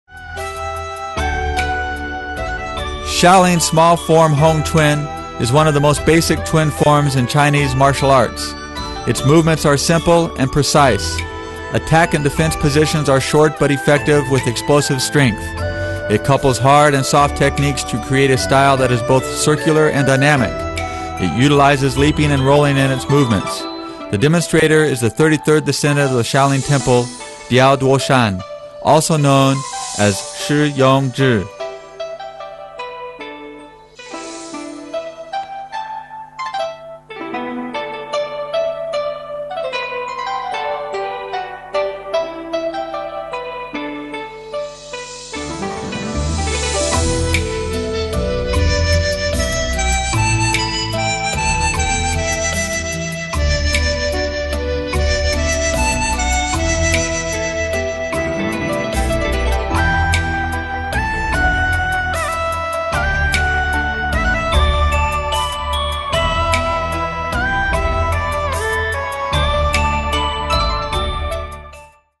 Shaolin Xiao Hong Quan Shi Yong Zhi 小洪拳
Shaolin Monk Shi Yong Zhi demonstrates one of the most traditional Shaolin routines practiced today.  This form is the epitome of Shaolin Kung Fu.